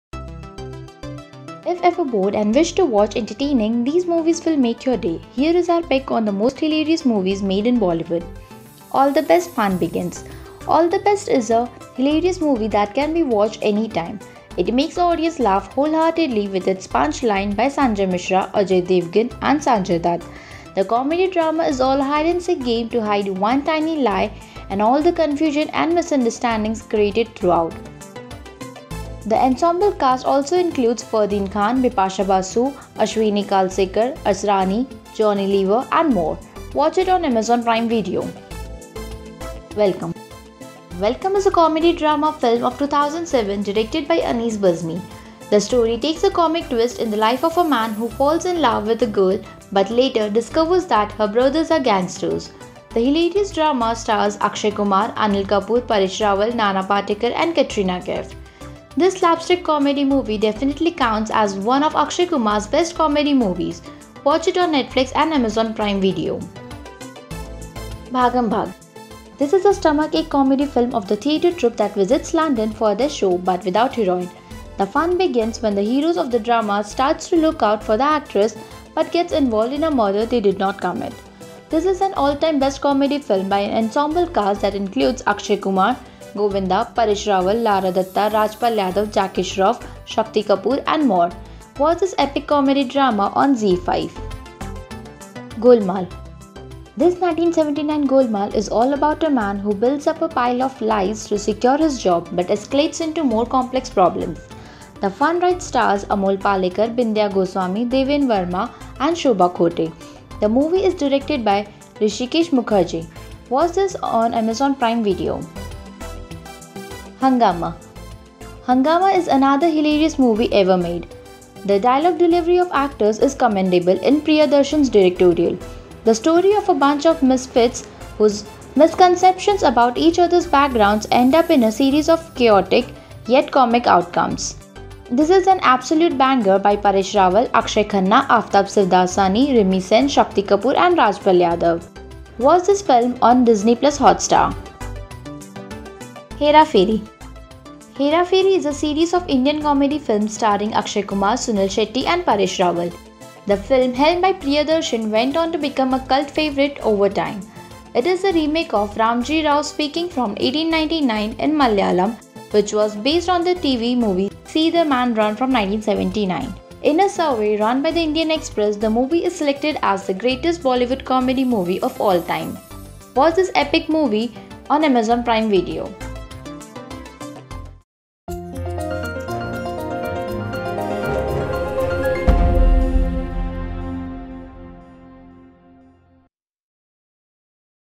ROFL And Stomach-Ache Hilarious Movies Ever Made In Bollywood | Lehren TV
If ever bored and wish to watch entertaining, these movies will make your day. Here is our pick on the most hilarious movies made in Bollywood.